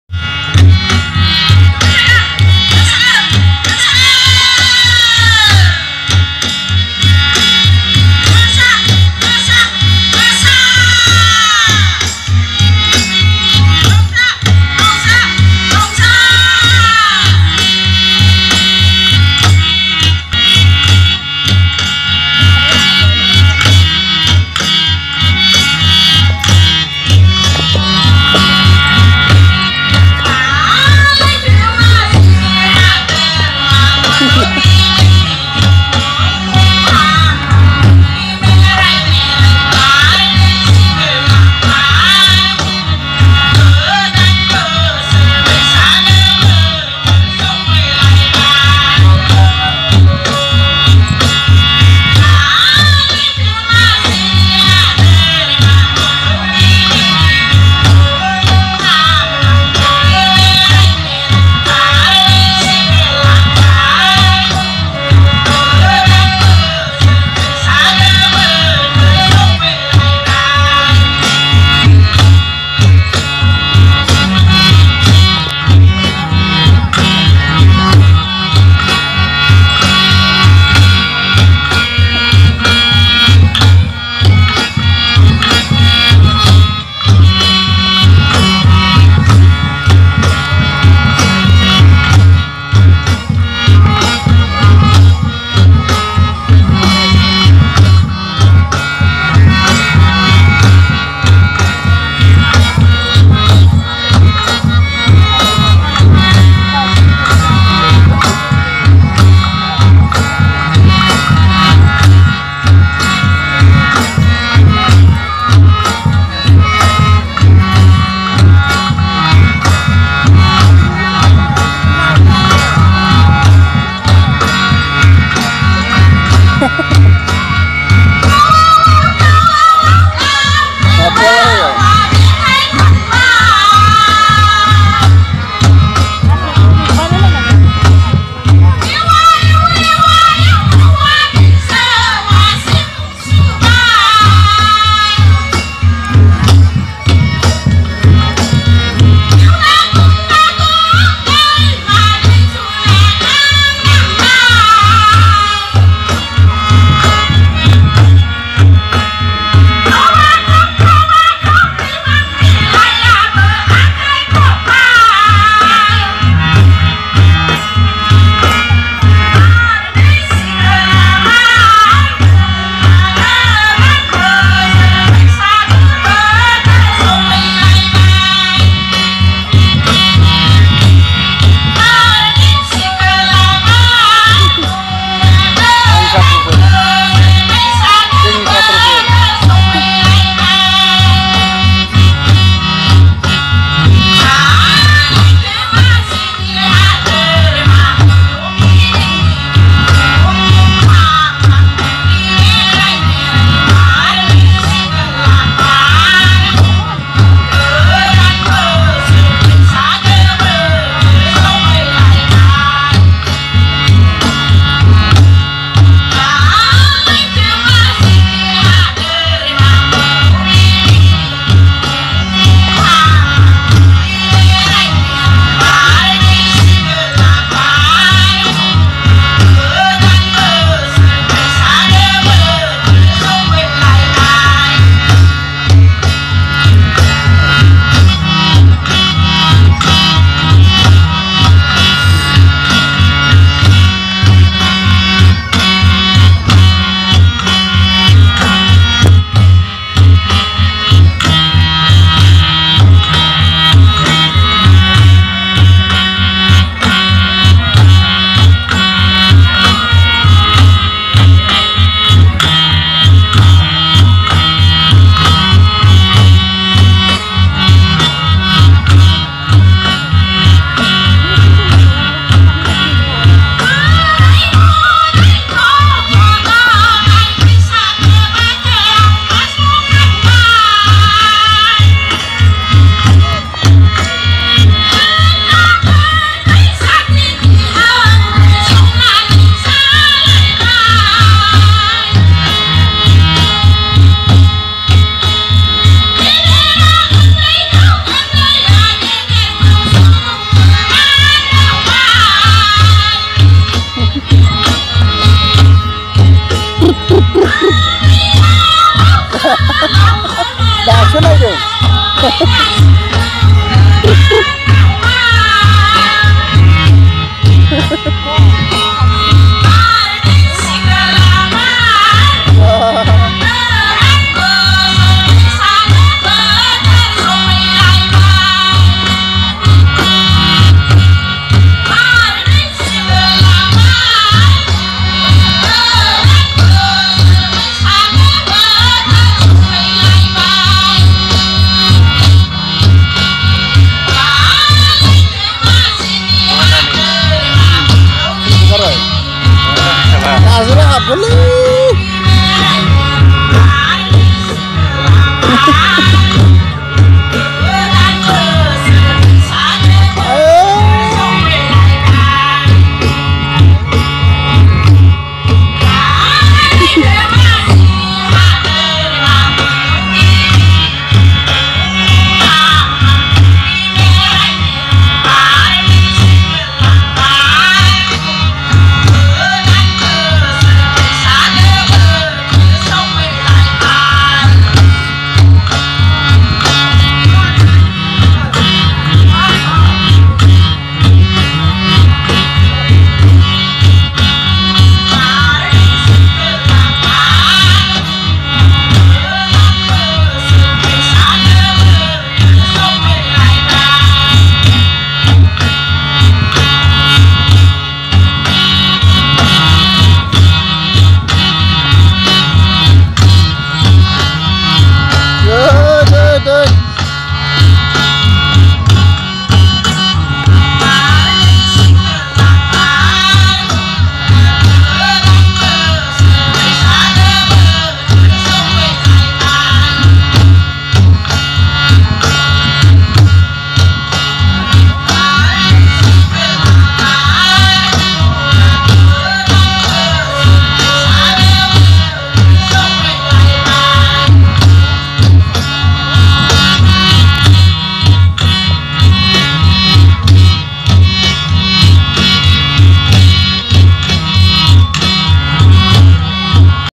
Beswargaon Bihamjw Group
Beswargaon Bihamjw Group dance, 12th Rangjali Bwisagu Fwrbw Celebration of Bijni Anchalik ABSU on 24 April, 2021 at Bodosa House Playground, Bijni